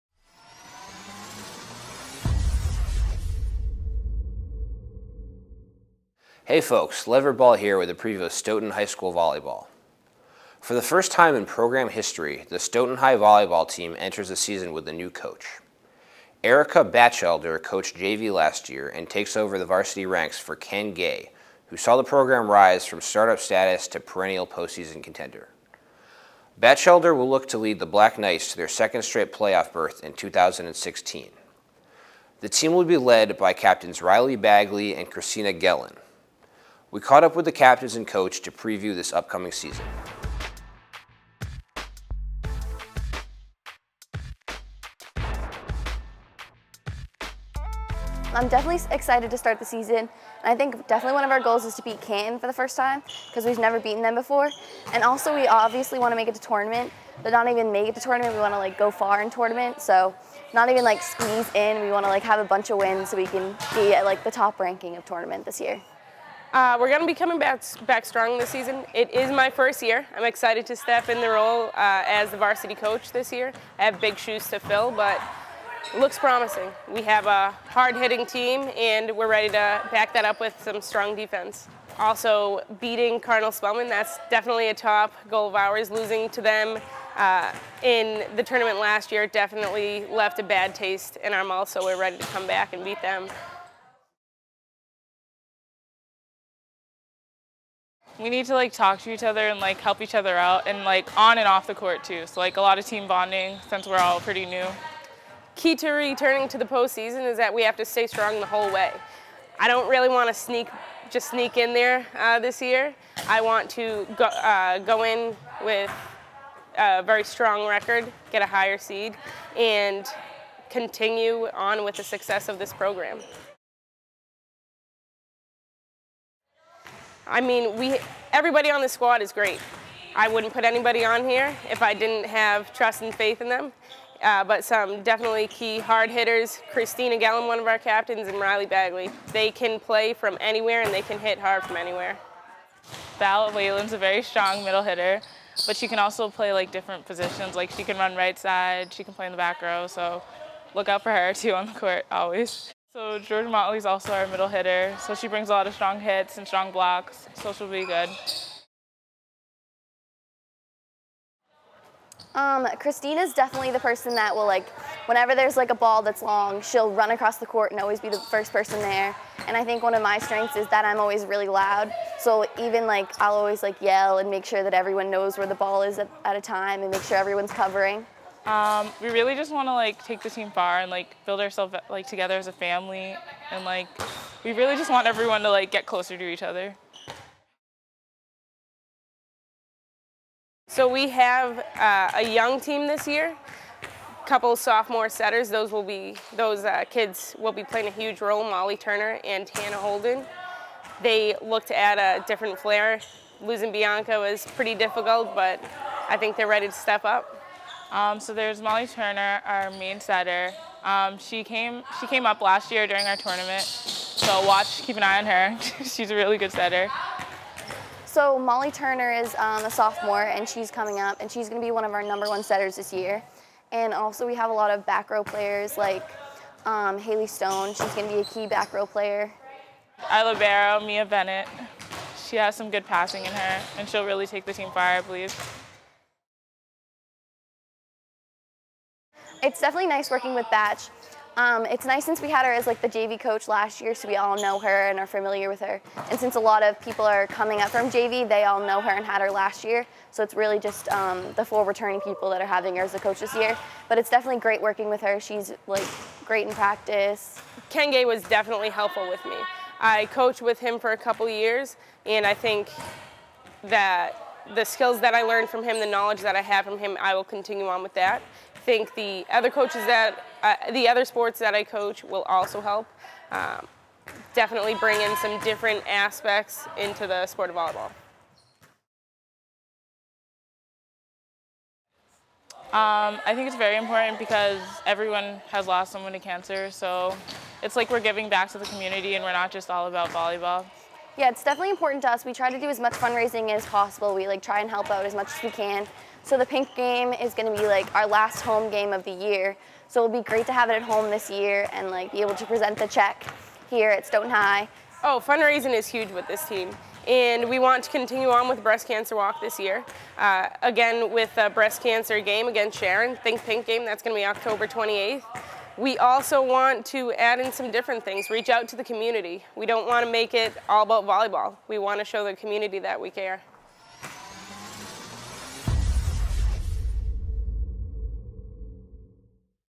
2016 Stoughton High Volleyball Season Preview
Stoughton High 2016 Fall Sports Previews: Volleyball - the Black Knights look to qualify for the postseason for the second straight year. Interviews with the team's new coach Erica Batchelder and captains Christina Gelin and Riley Bagley.